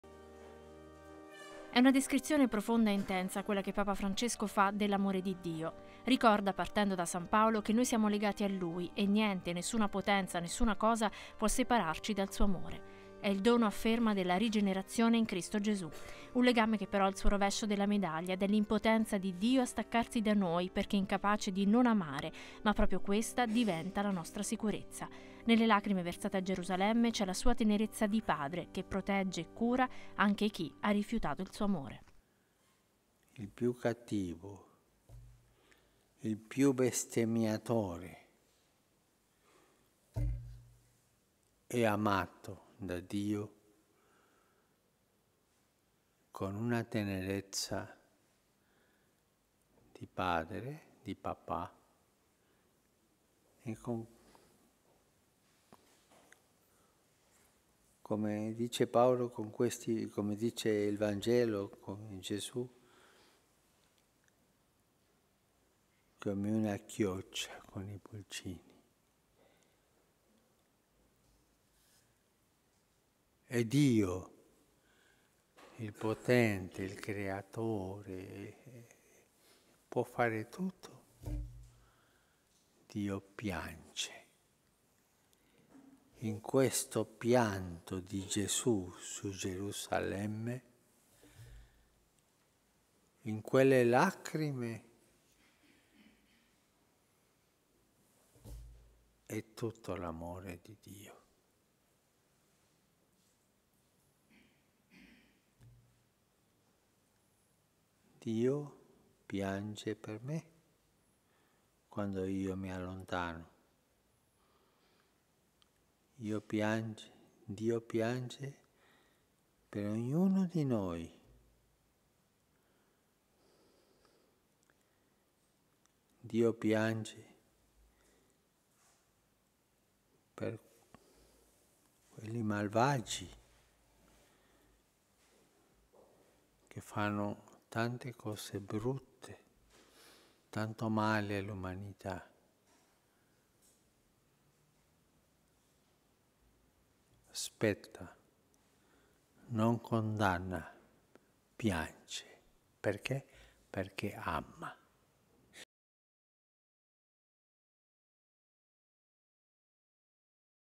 Papa messa Santa Marta 29-10-2015
Papa Francesco: il dono dell’amore di Dio ci rende vincitori

Nell’omelia della messa a Casa Santa Marta, il Papa ricorda che Dio non può non amare e che il rifiuto degli uomini lo fa piangere, in quelle lacrime – ricorda  - c’è tutto il suo amore.